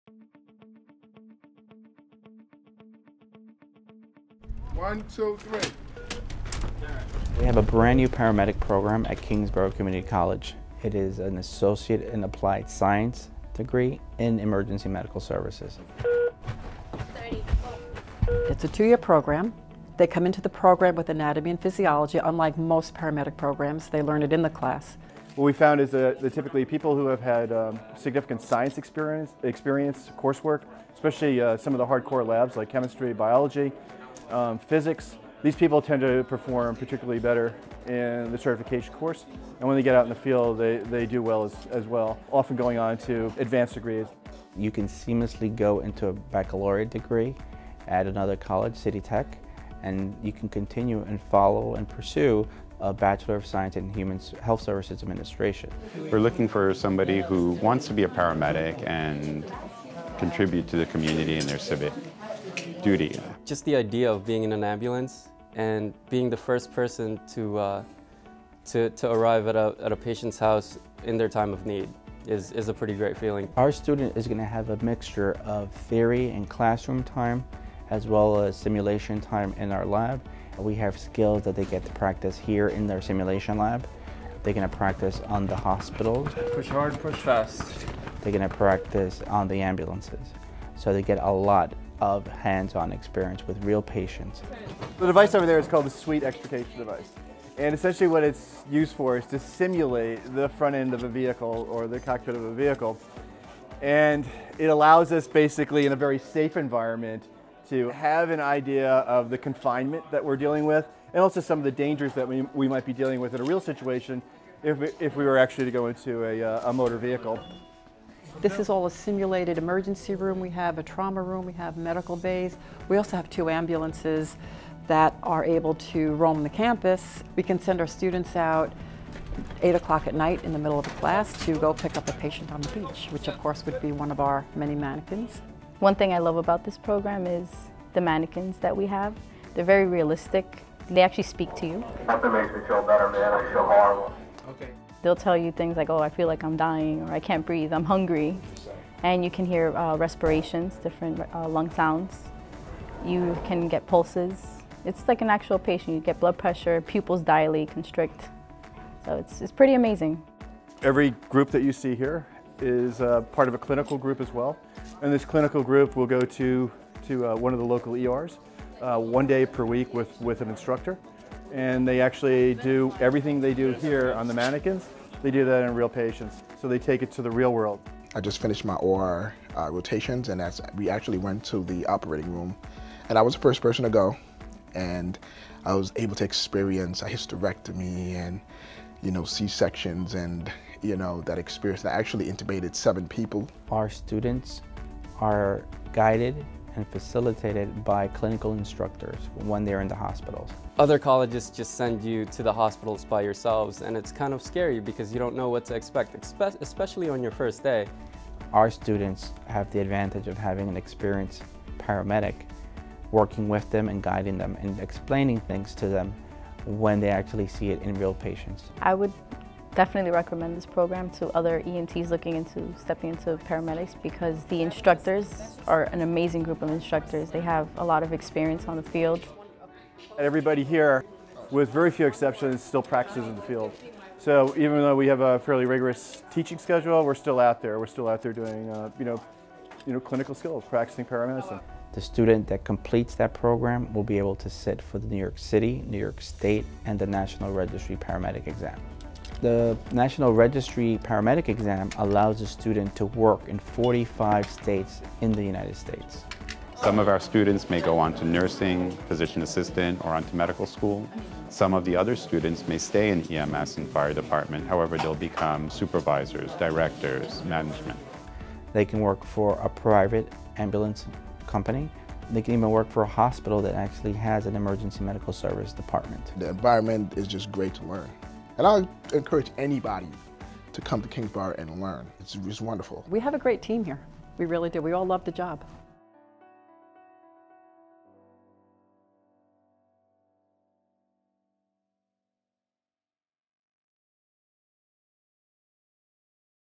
CUNY Kingsborough EMS Paramedic Program
Our intensive 2-year program leads to certification as a paramedic in NYC and NYS.  You'll learn to deliver advanced pre-hospital care in a state-of-the-art Simulation Center with high-fidelity training manikins; in hands-on clinical rotations at area hospitals; and aboard our own ambulance.